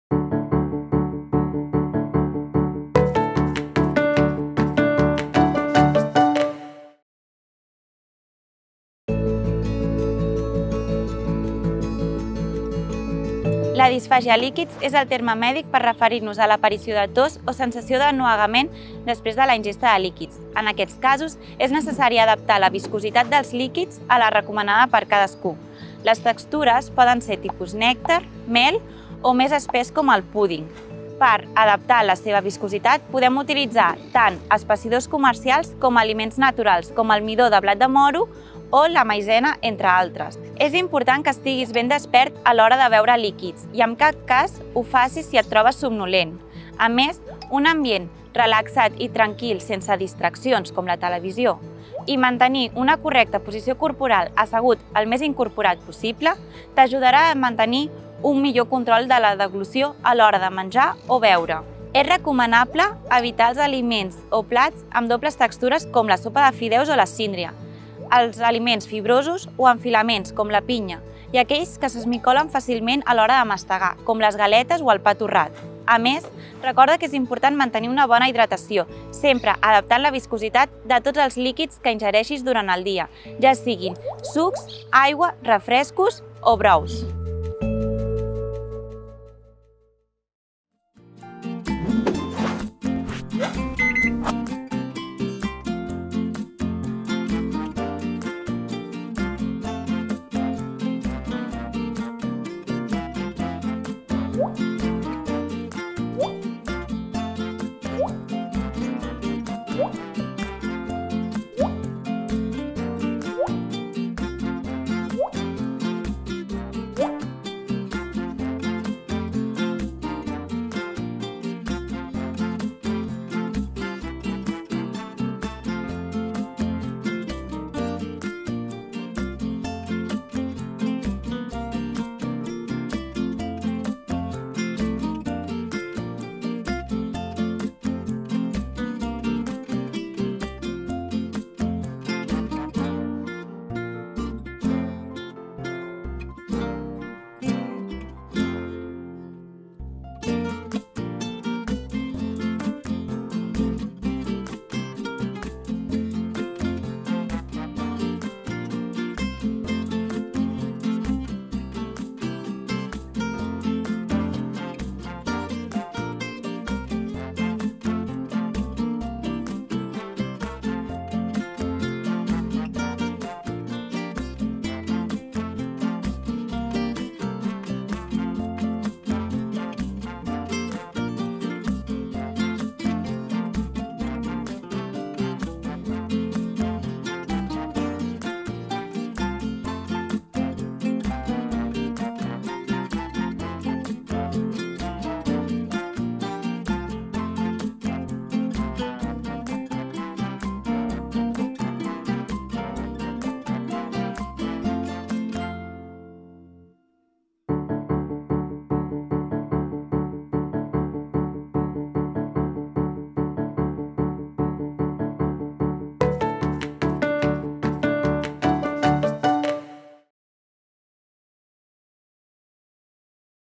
Què i com menjar durant el càncer. Disfàgia a líquids + Guacamole
Recepta de guacamole indicada per si es presenta disfàgia a líquids, és a dir, dificultat per empassar líquids durant el tractament del càncer. Elaborada per l'Institut d'Investigació Sanitària Pere Virgili. La recepta va acompanyada amb les recomanacions d'una dietista-nutricionista de l'Institut Català d'Oncologia.

El projecte és una producció de la Xarxa Audiovisual Local i elaborat en col•laboració amb institucions de referència en oncologia, nutrició i recerca com la Fundació Alícia, l'Institut Català d'Oncologia (ICO), l’Institut d’Investigació Biomèdica de Bellvitge (IDIBELL), l’Institut d’Investigació Sanitària Pere Virgili (IISPV) i la Universitat de Barcelona (UB). També hi han col•laborat: Les Moles Restaurant, Expogestió i el Mercat Municipal de Molins de Rei.